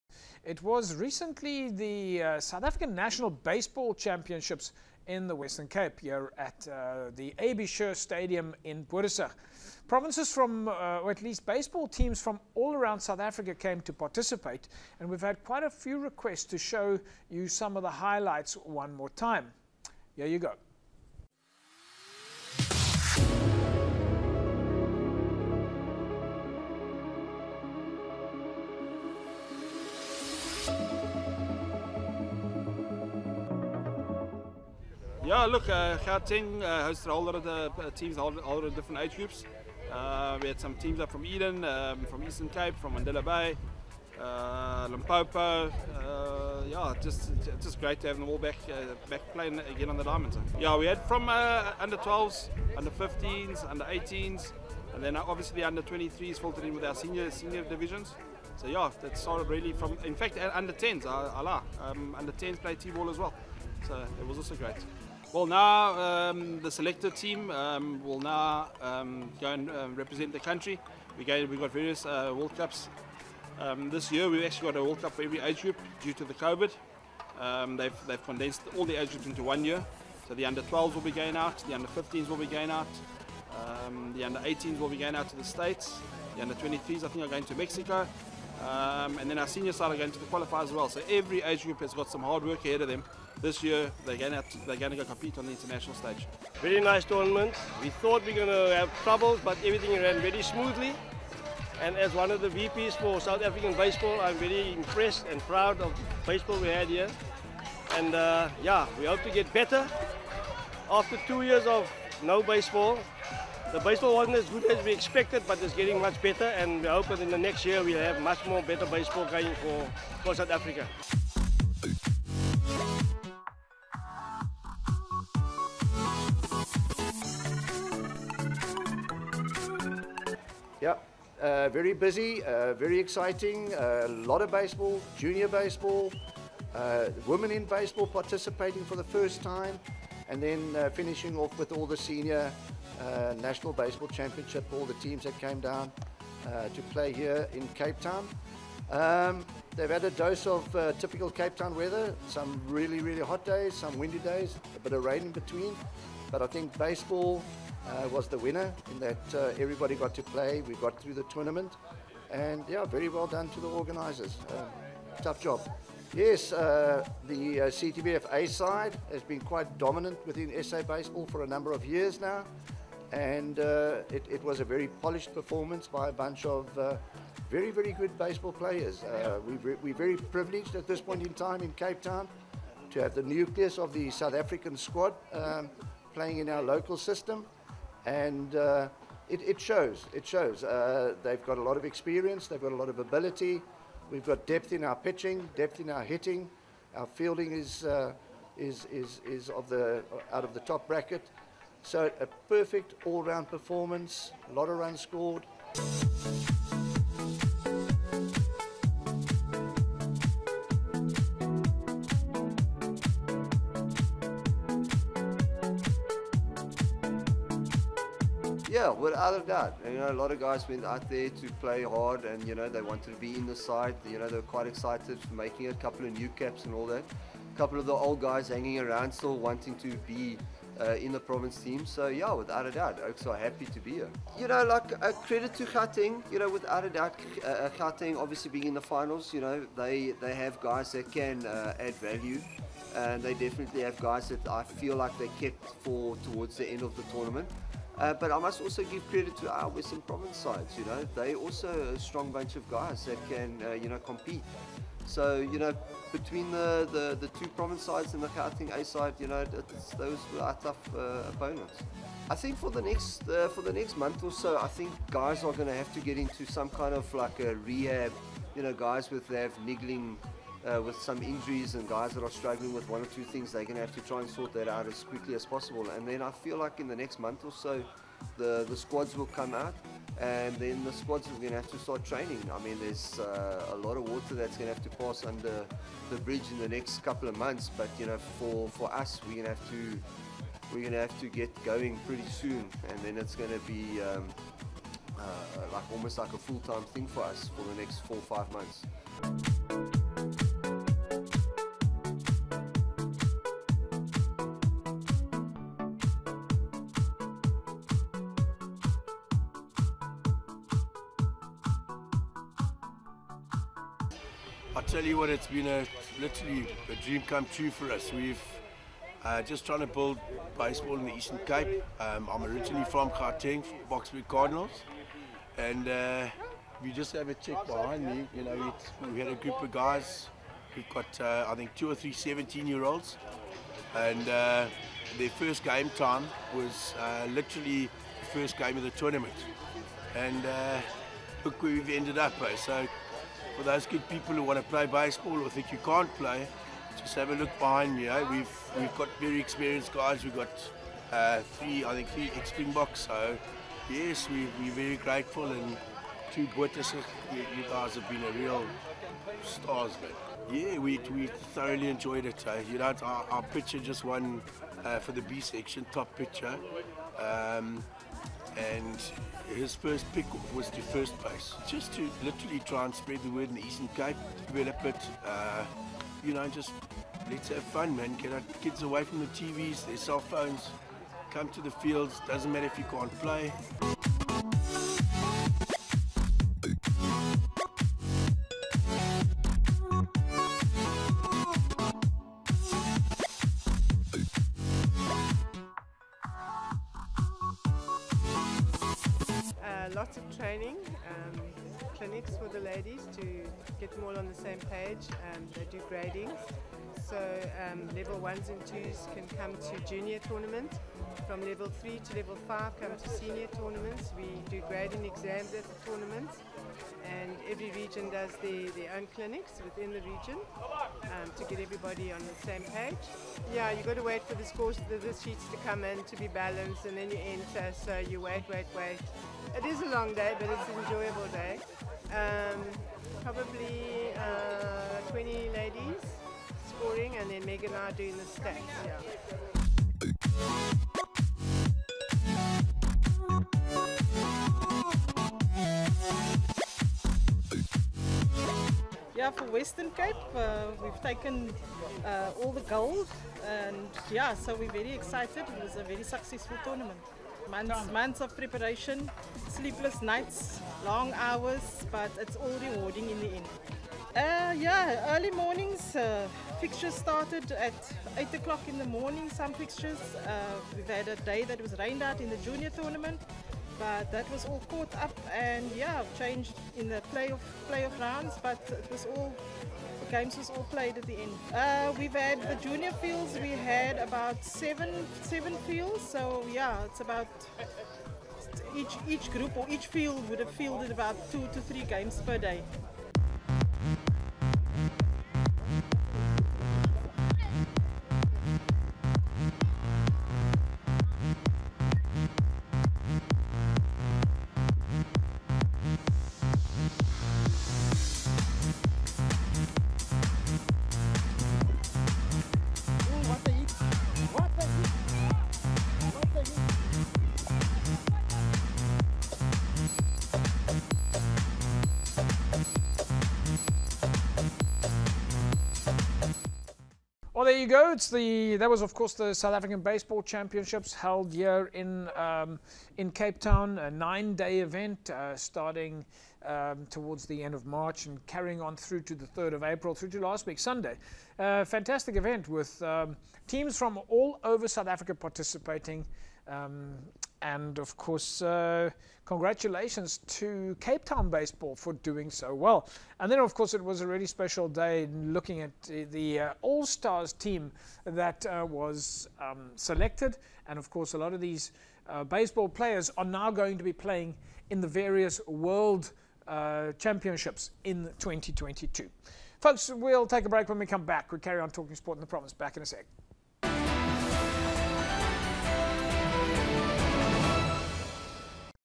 Western Cape hosts South African Baseball National Championships 2022
Sport Federation TV is joined weekly by Sport Federations in the Western Cape. Flights weekly on Cape Town TV DStv Ch 263. Fri 1pm, Sat 8am. DSTV Ch263